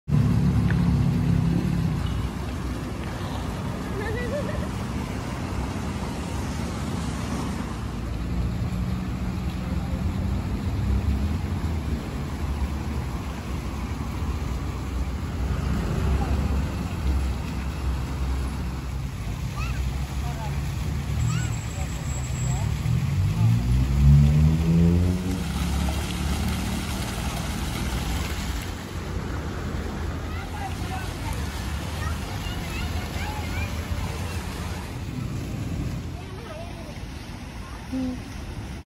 💡 Lampu klip - klip ala E-Tron. Menikmati pandangan lampu neon di Taman Muhibbah Labuan 🇲🇾
Bawa keluarga pusing bandar.